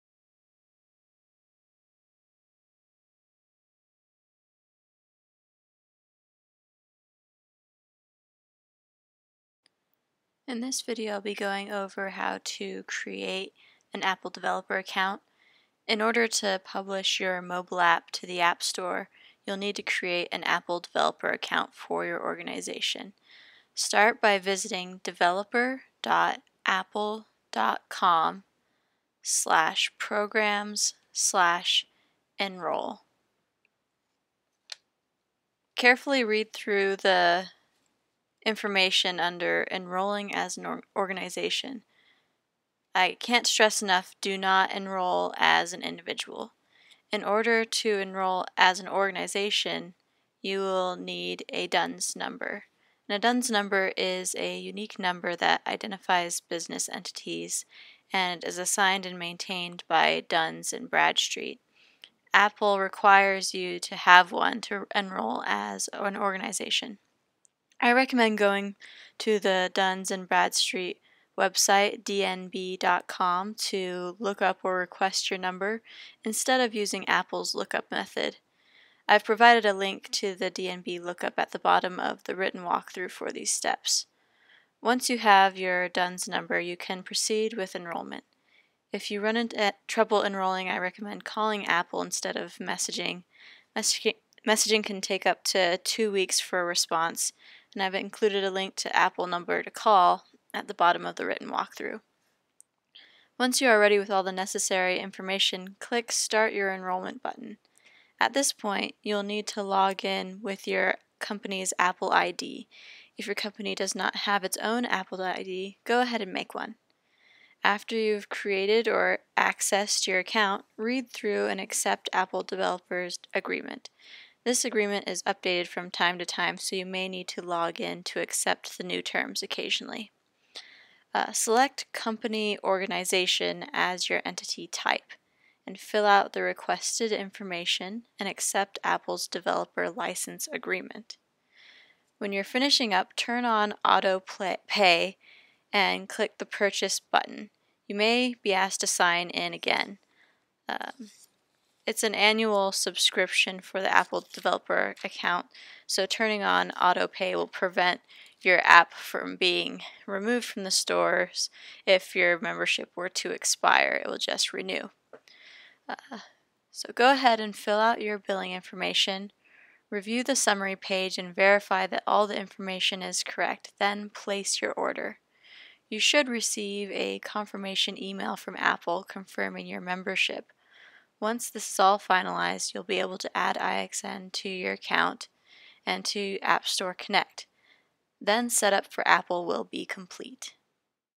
Apple Developer Enrollment Walkthrough
This step is necessary to place your app in the store, and for us to be able to build, test, and distribute your app for you. This walkthrough outlines the enrollment process for an Organization in the Apple Developer Program